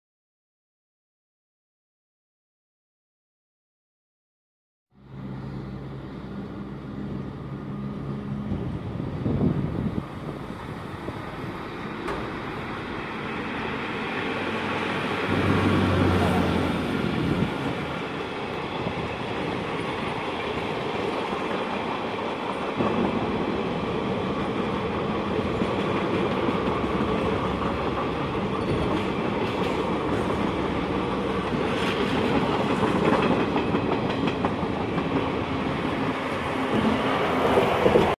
Diesel engine deteriorated, then electric engine rescue the train | Indian Railways
Dead Itarsi WDP4D then Vadodara WAP4 rescue the 11273 Itarsi Katni Express Train.

Kareli,wcr

Date Of video - 15/03/2020
Time - 11.25